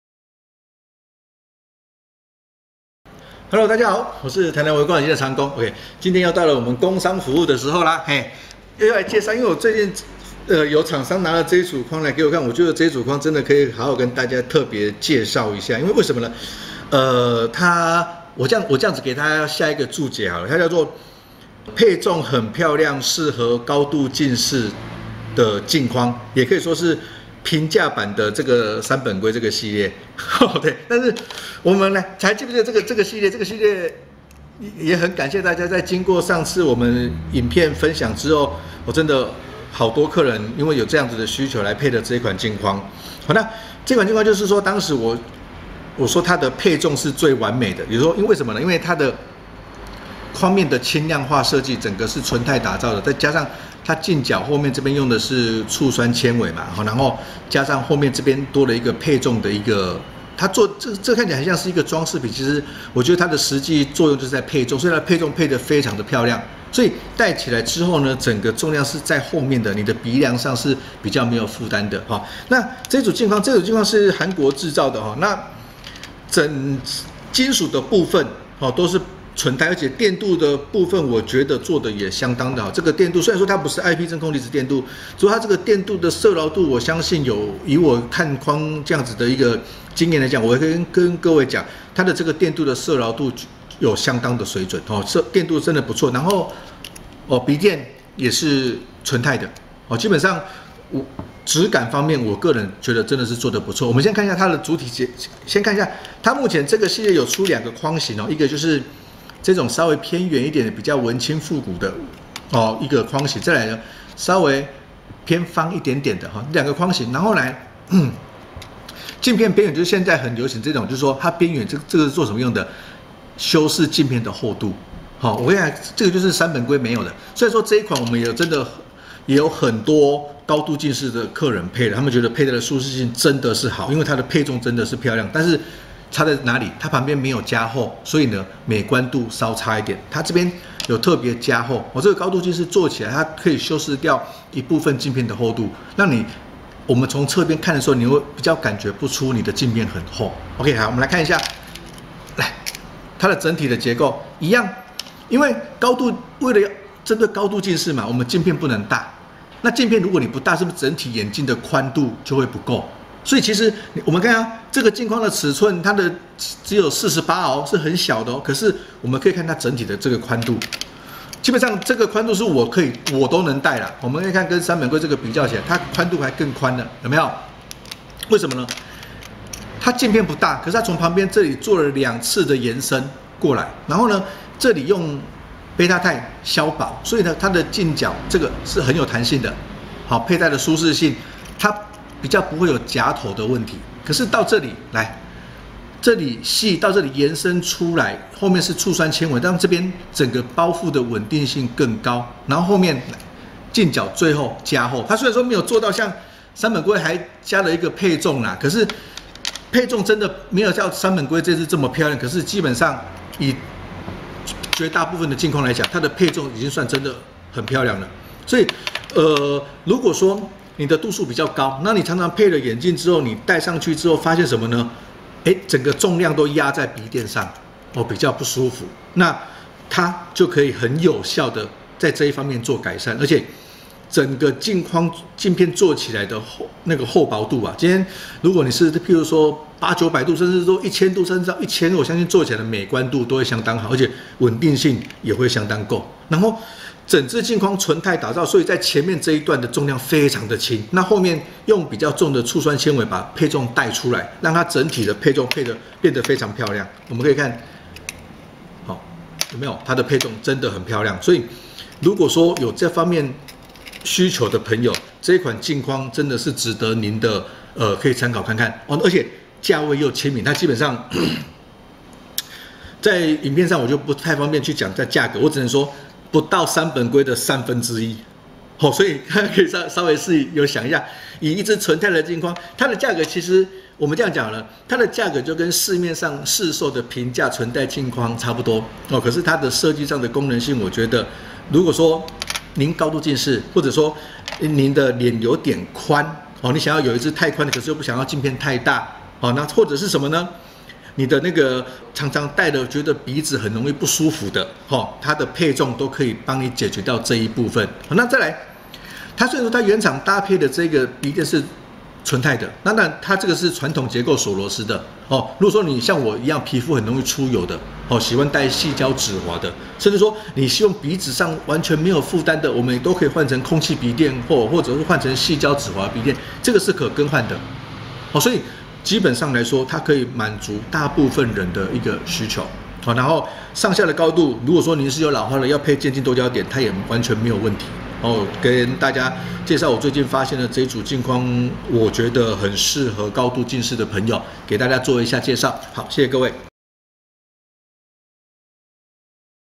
【產品分享及開箱文】504~平價版的杉本圭！？配重漂亮又適合高度近視的鏡框介紹
不只是配重漂亮
鏡框的厚度也有很大的修飾效果
高度近視的人非常合適哦！
一起來聽長工介紹吧！

任何問題歡迎私訊
✅唯光眼鏡粉絲團